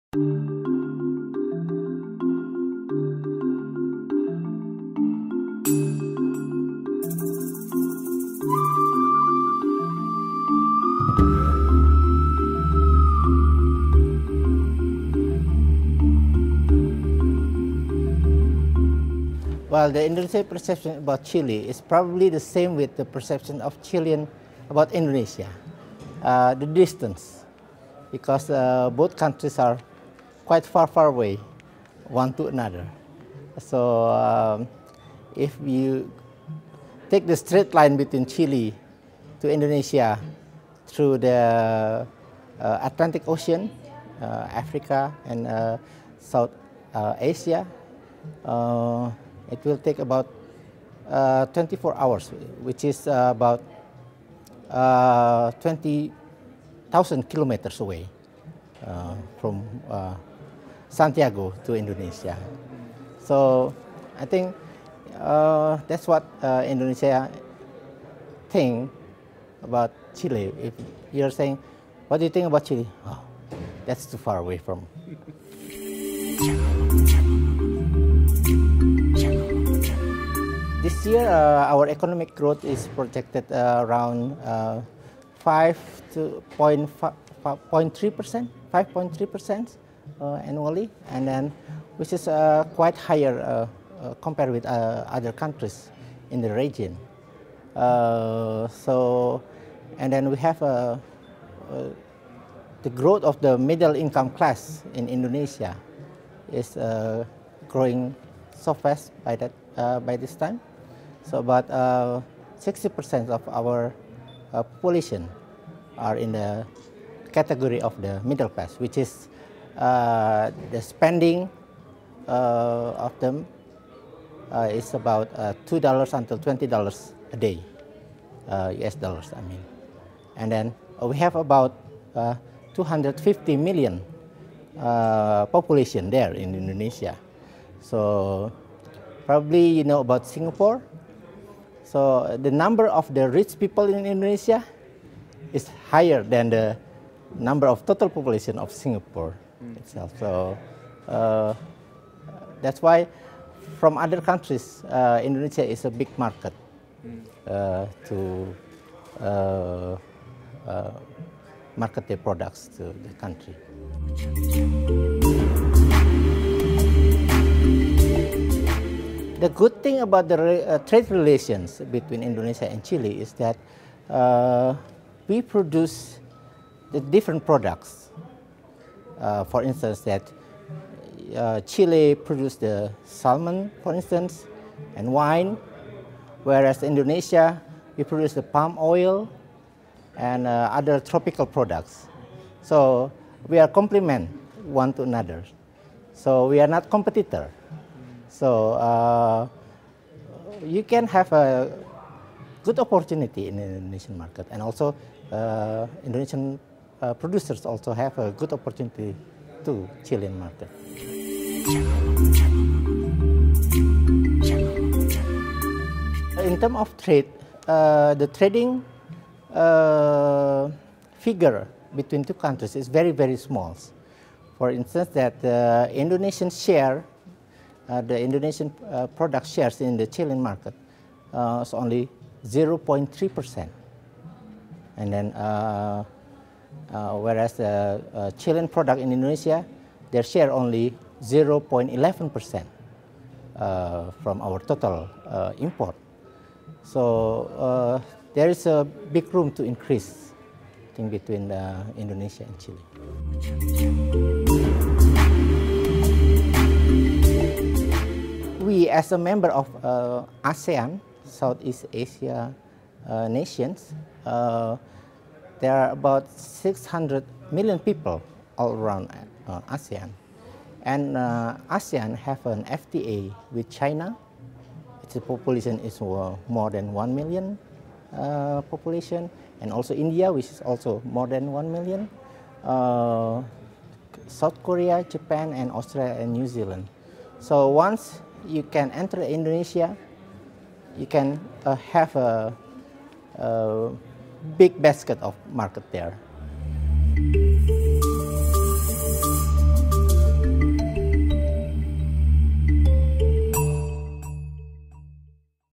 Entrevista Ari Satria, director de mercados, Ministerio de Comercio Indonesia
En su visita a nuestro país, el director de Mercados y Exportaciones del Ministerio de Comercio de Indonesia, Ari Satria comentó las oportunidades en la relación bilateral con Chile.  Más información en el Portal Chile Asia Pacífico